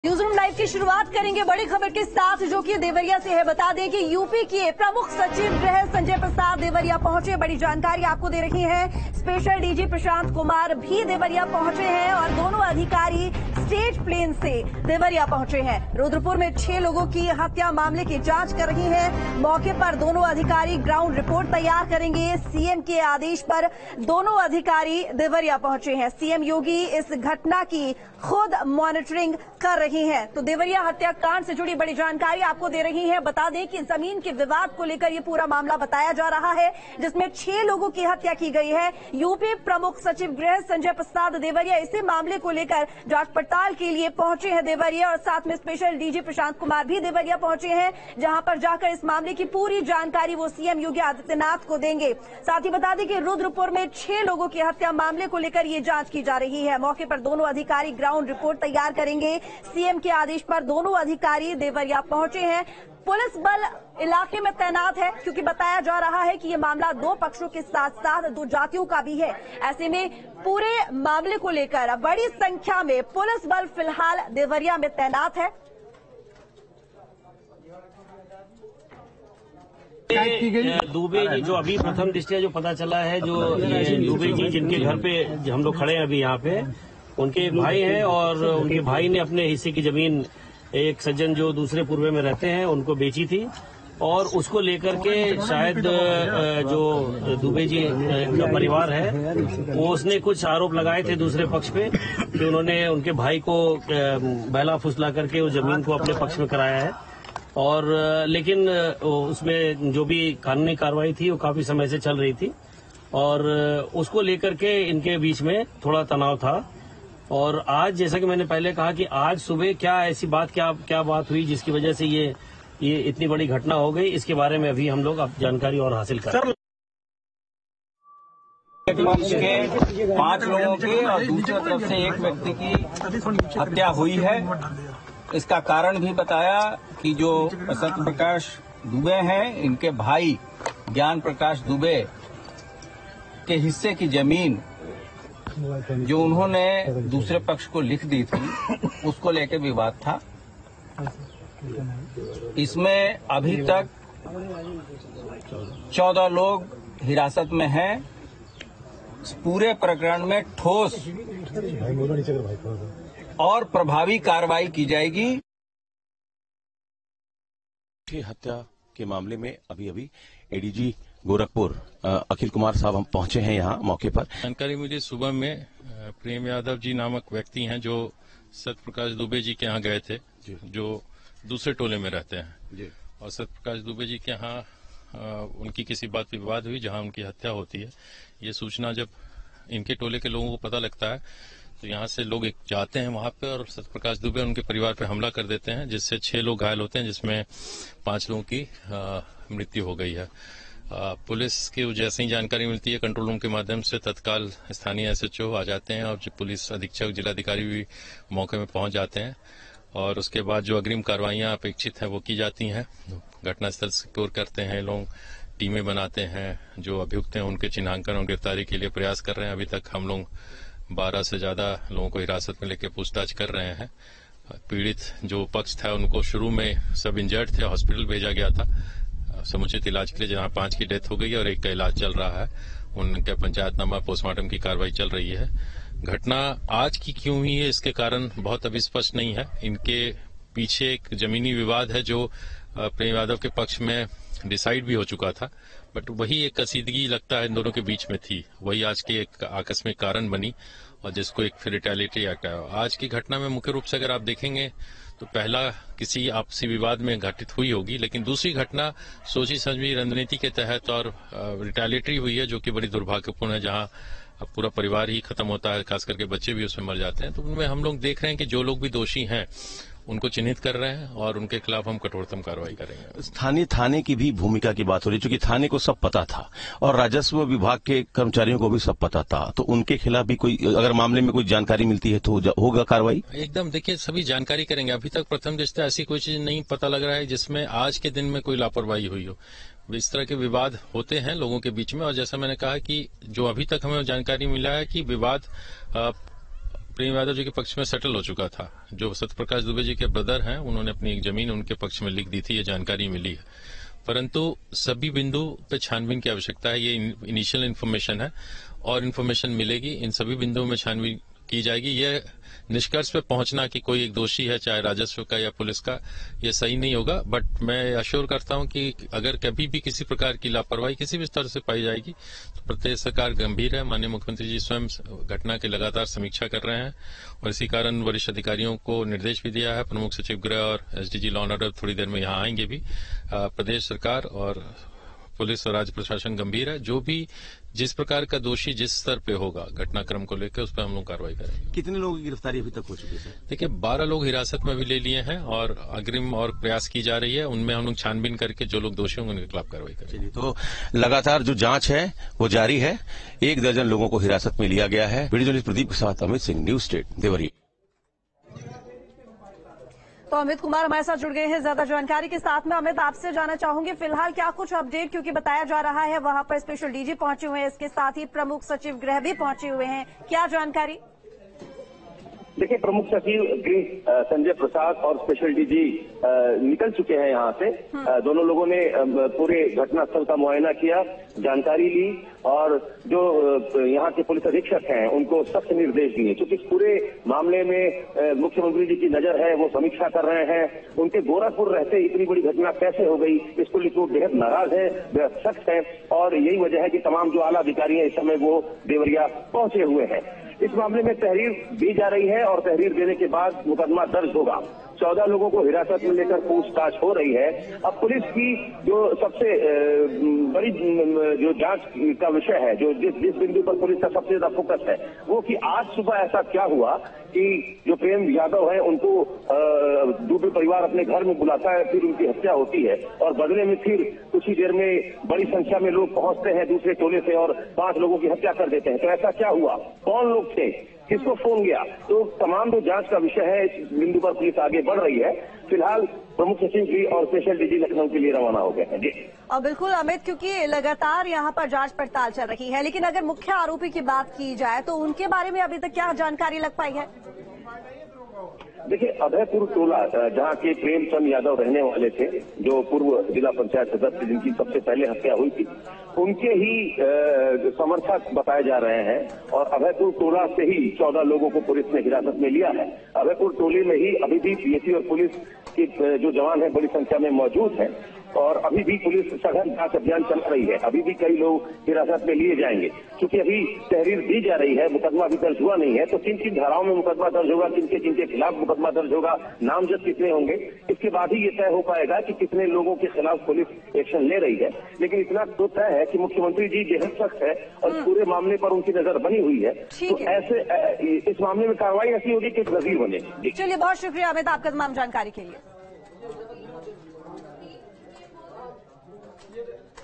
Deoria Case Update: प्रमुख सचिव गृह Sanjay Prasad आला अधिकारियों के साथ घटनास्थल पर पहुंचे |Breaking
देवरिया: UP प्रमुख सचिव गृह संजय प्रसाद देवरिया पहुंचे
स्पेशल डीजी प्रशांत कुमार भी देवरिया पहुंचे
दोनों अधिकारी स्टेट प्लेन से देवरिया पहुंचे
रुद्रपुर में 6 लोगों की हत्या मामले की जांच कर रहे हैं
मौके पर दोनों अधिकारी ग्राउंड रिपोर्ट तैयार करेंगे
सीएम के आदेश पर दोनों अधिकारी देवरिया पहुंचे
CM योगी इस घटना की खुद मॉनिटरिंग कर रहे

Deoria: UP Principal Secretary Home Sanjay Prasad reached Deoria
Special DG Prashant Kumar also reached Deoria
Both the officers reached Deoria by state plane
Investigating the murder of 6 people in Rudrapur
Both officers will prepare ground report on the spot
On the orders of CM, both the officers reached Deoria
CM Yogi himself is monitoring this incident

News State Apps डाउनलोड करें:
Download Android and iOS Apps for more updates: